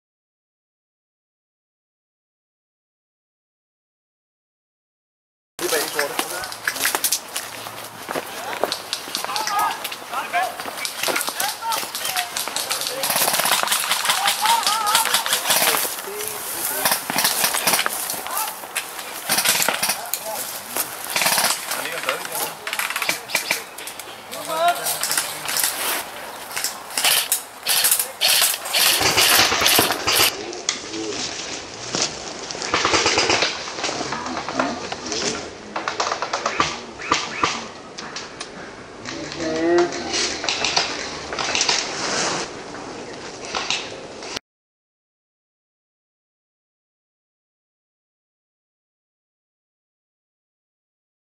Dude, you forgot you eye protection ! - In an airsoft game...
Please, don´t ever do this in the middle of an airsoft game.